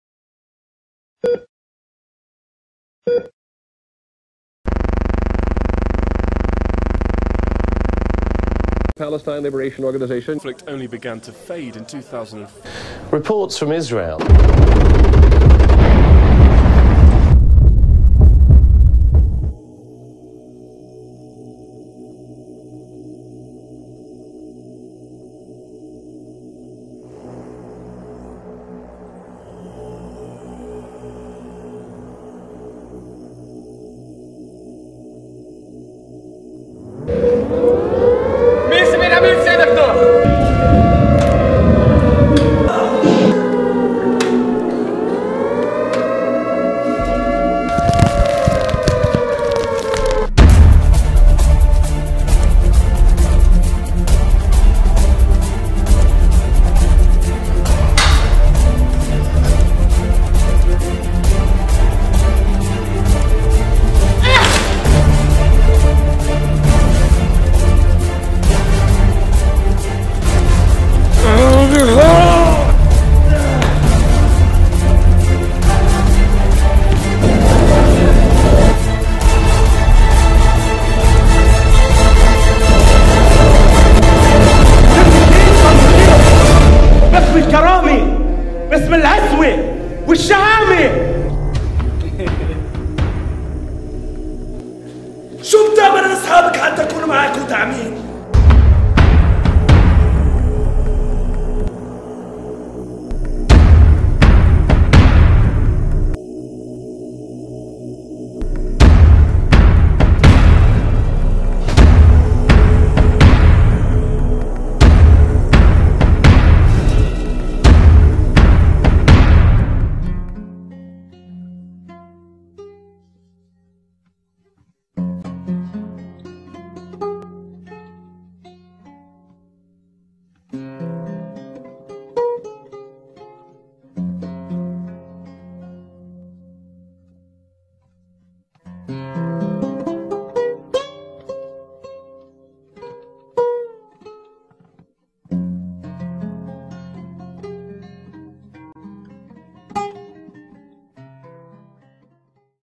Suicide Note from Palestine
Amal, embodying her own country, Palestine, is lying in a hospital bed. Her heartbeat has stopped and medical staff wearing white UN-marked coats are trying to resurrect her. Amal has decided to die as a political act. Suicide Note from Palestine, originally produced in 2013 and performed to wide acclaim in Jenin Refugee Camp, is re-staged in December 2014 and performed in Jenin, Bethlehem and Hebron as well as live streamed to an international audience. In September 2015, the play will come to New York.